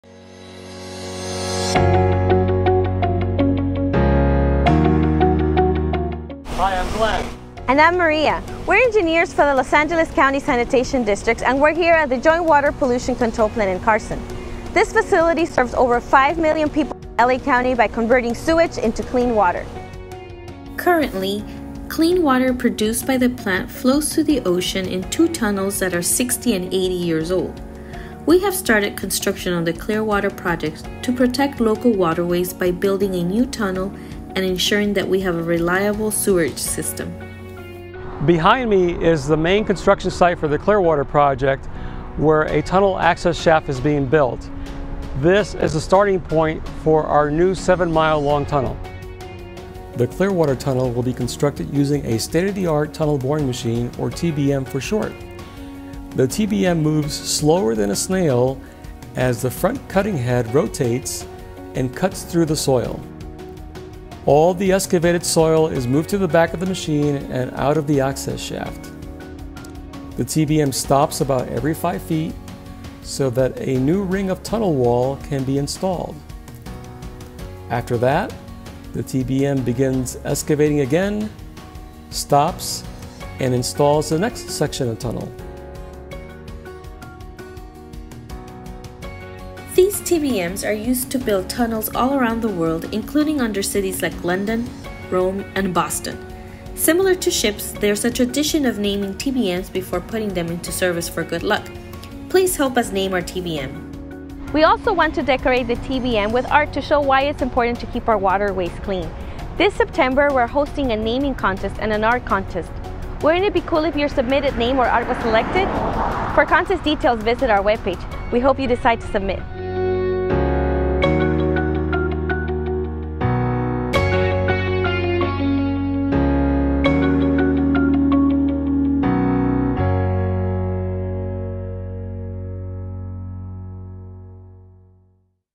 The Clearwater Project - TBM Naming & Art Contests
The Los Angeles County Sanitation Districts invite Los Angeles County students in grades 1 through 12 to help us name the Clearwater Project tunnel boring machine (TBM) before it starts its quiet journey to build a new tunnel that will protect our local waterways. We also need your help to decorate the TBM with your artwork. To participate in our TBM naming and art contests, please see details below. Winners will receive recognition and cool prizes.

Create artwork that shows why it is important to keep our rivers and ocean clean.

Three artwork winners will be selected - one for elementary school, one for middle school and one for high school. The winning works of art will be placed on the TBM.

The artwork you submit can be a painting or drawing, either created by hand or on a computer. Rules:

• Your entry can be letter size (8½" x 11"), tabloid (11" x 17") or in between.
• Your entry must be your original art.
• One entry per student.
• You must be a student in Los Angeles County between grades 1 and 12 to enter.
• Submit your entry between September 15 and October 15, 2020.